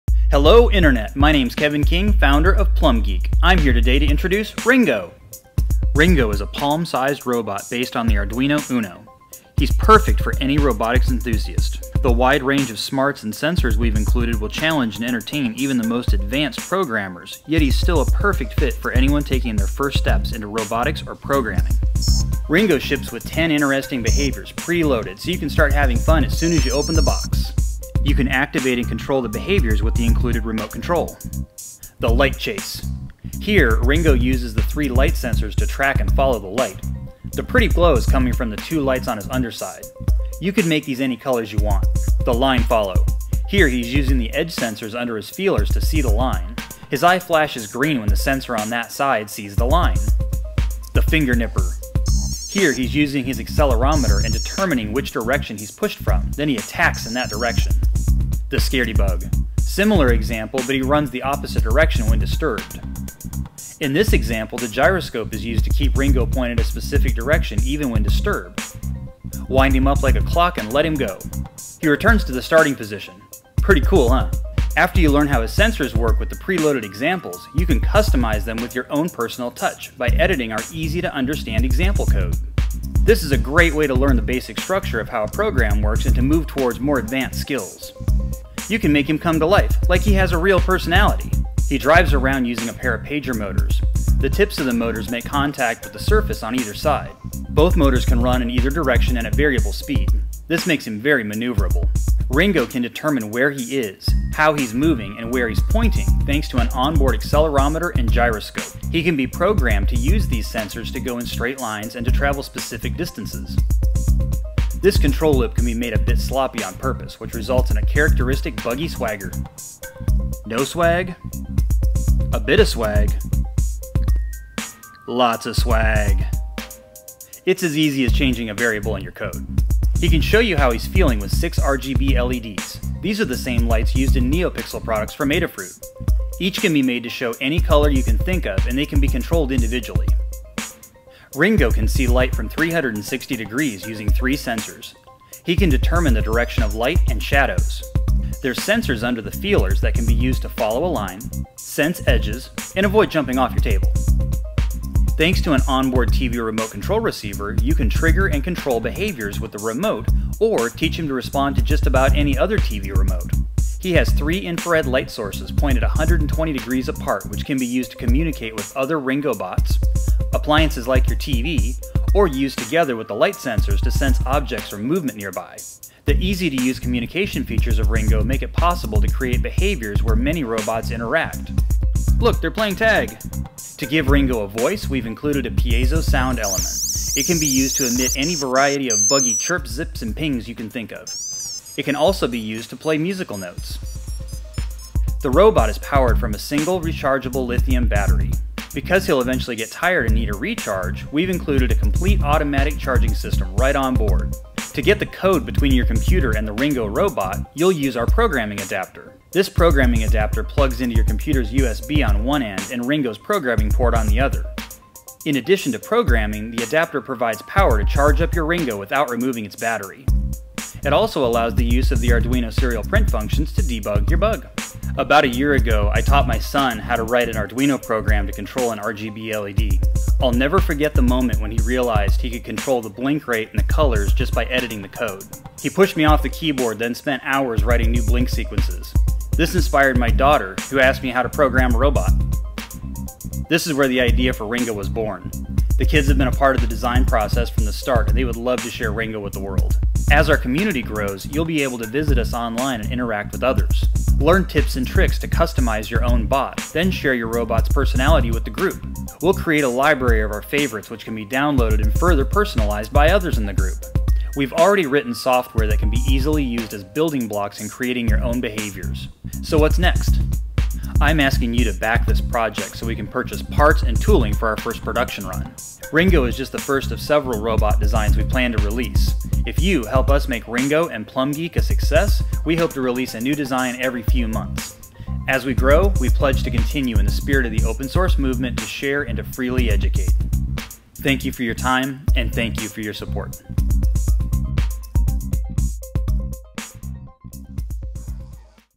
Ringo - The hackable palm size robot
Introducing Ringo the hackable robot, programmed with Arduino.  Start a swarm of your own!

Ringo was crowdfunded on Kickstarter in Spring of 2015.